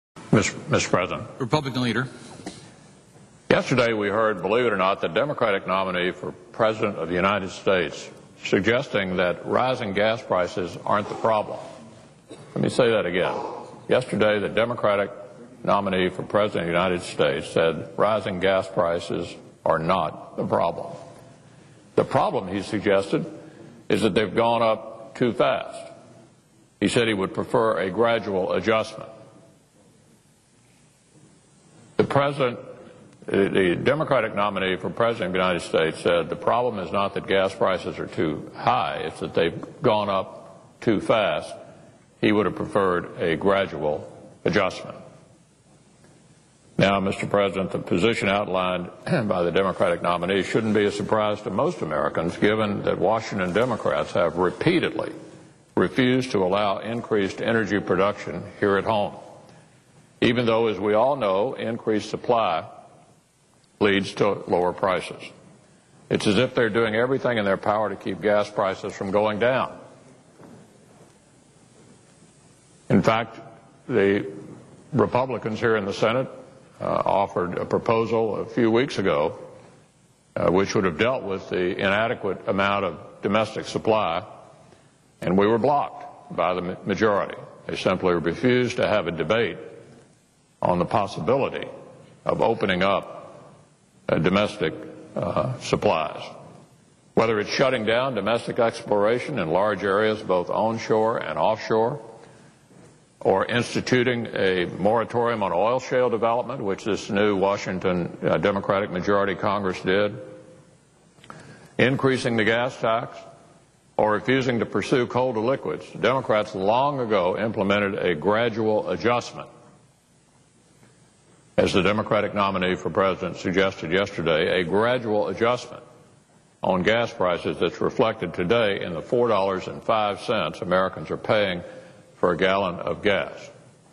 McConnell Criticizes Obama's "Gradual Adujstment"
Senate Republican Leader Mitch McConnell criticizes Sen. Barack Obama (D-IL) for saying he "would have preferred a gradual adjustment" when asked if higher gas prices are a good thing.

McConnell also criticizes Senate Democrats for blocking domestic energy production in the United States.